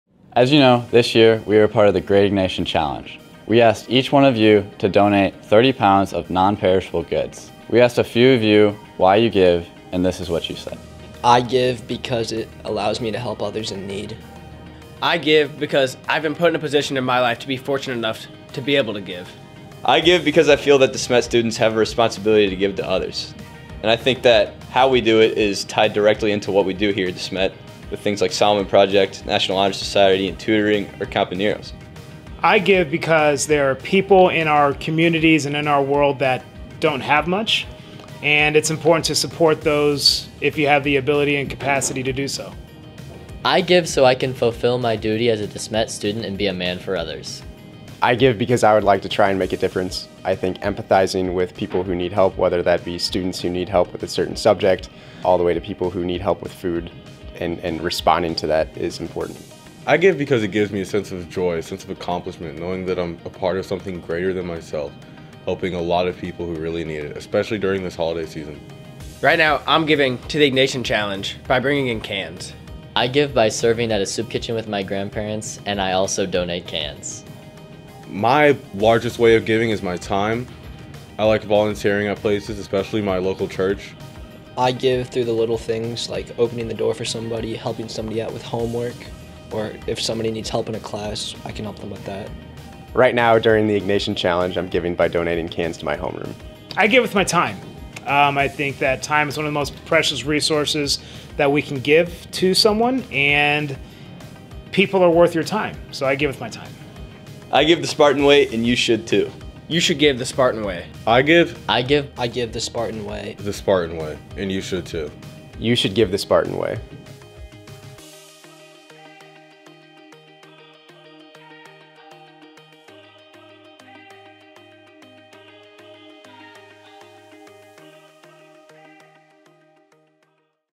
Why We Give - TheSpartanWay - Video by Kalle Evjen '23
Students and teachers from De Smet Jesuit High School share why they give to the Great Ignatian Challenge and elsewhere as part of their mission to be men for others and give the Spartan way.